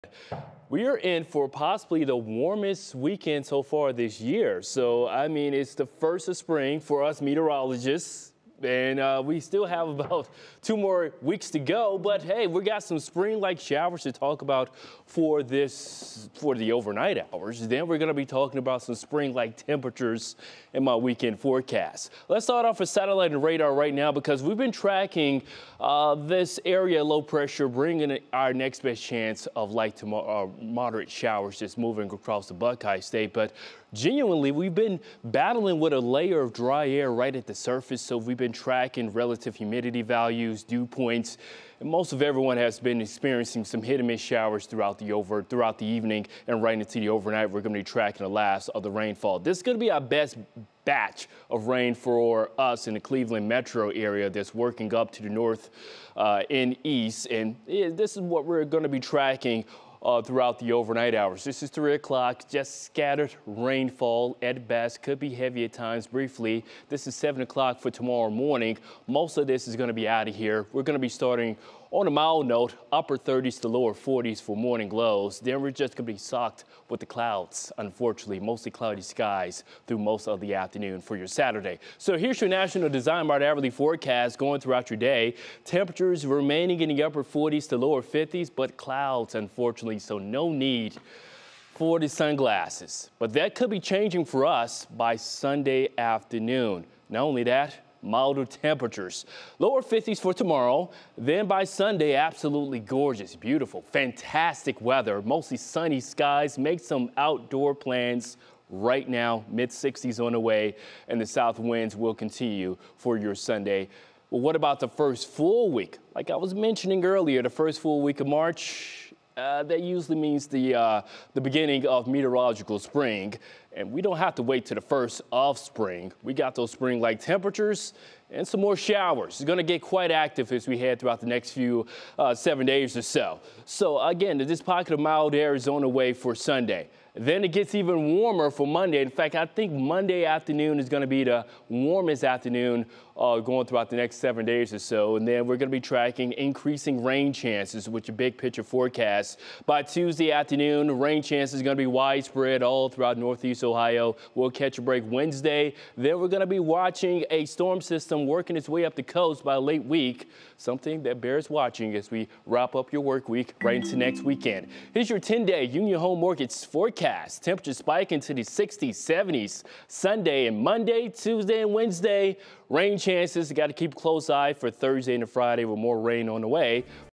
Cleveland area weather forecast: Spring-like showers overnight
Your 11 p.m. Greater Cleveland and Northeast Ohio weather forecast for March 1, 2024: For this weekend a pretty big storm system is slamming the West Coast with some incredible snow. Cold air is the culprit, as a new air mass makes its way south from Alaska.

It may all seem pretty confusing but basically think of cold air masses and warm air masses like water and oil. They don't always mix too well. A big movement of cold air will push warmer air around.

So when we have cold air that advances south, we'll get warmer air that is shoved north.

This time of year that is FINE by US! But since we are early in the season these pops of heat are often short lived.

Gradually the warmer air wins out as the days get longer and the sun gets to a higher angle in the sky.